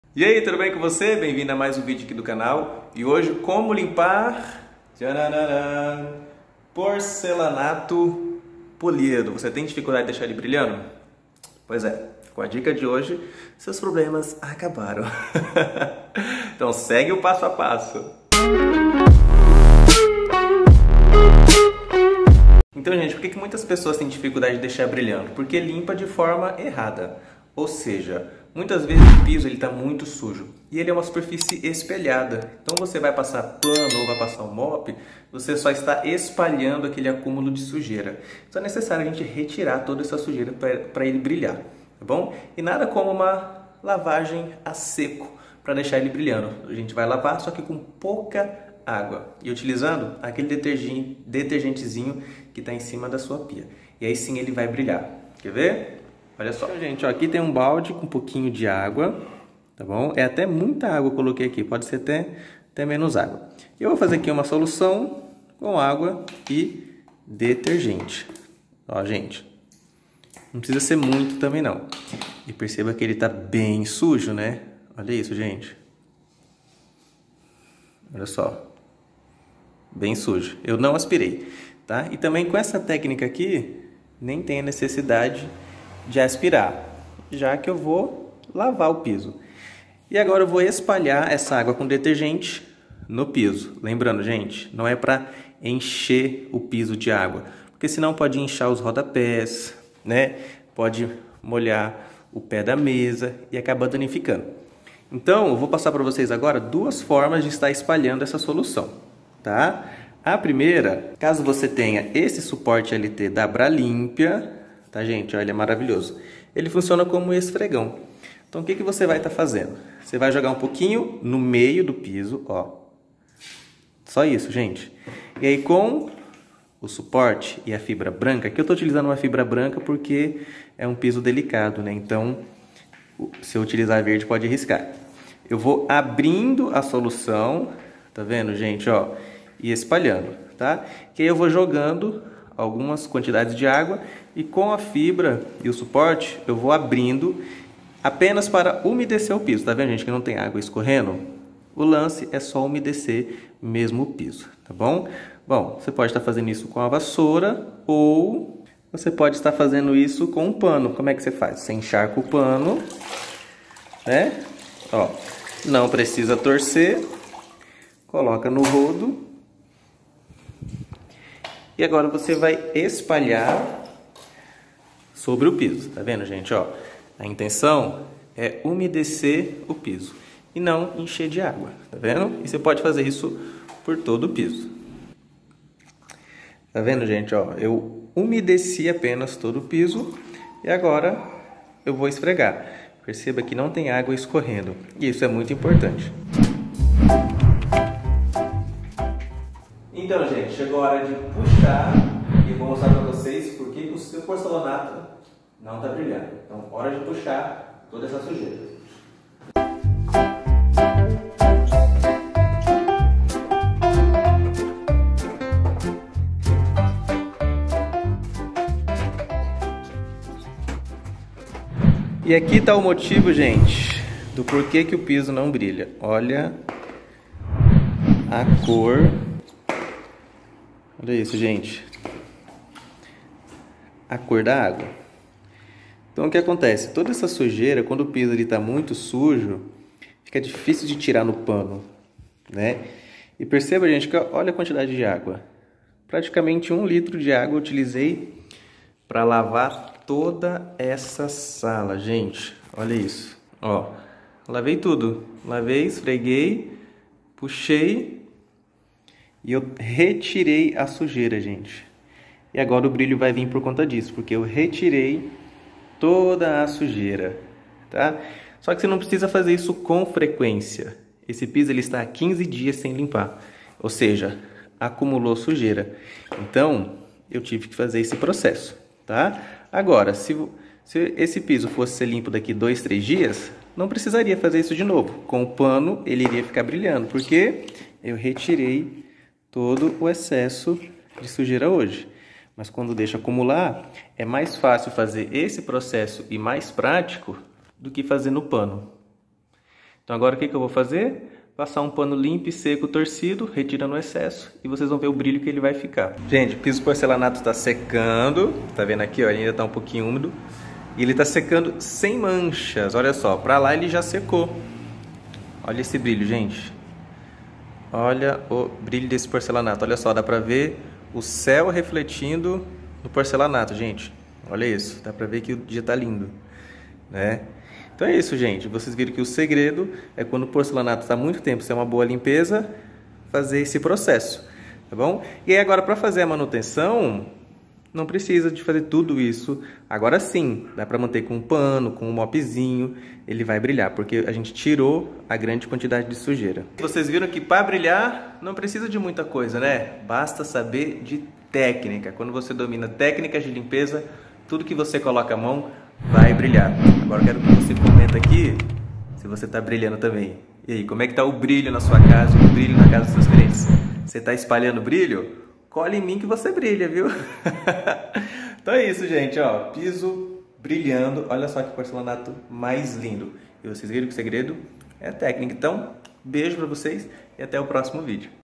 COMO LIMPAR PORCELANATO POLIDO E DEIXAR BRILHANDO
Quer aprender como DOBRAR o valor de sua diária e ganhar de 3 à 6 mil reais COMO DIARISTA aqui no Brasil?

Muitas pessoas tem dificuldade de deixar o porcelanato polido brilhando, mas com a técnica de hoje você vai deixá-lo brilhando com apenas um detergente. Isso porque por ser uma superfície polida, quando muito sujos necessitam ser lavados, mas não significa jogar baldes de água. Veja e aplique na sua casa ou de seu clientes esse tipo de lavagem à seco, super prático e eficiente, e sem riscos de danificar os móveis.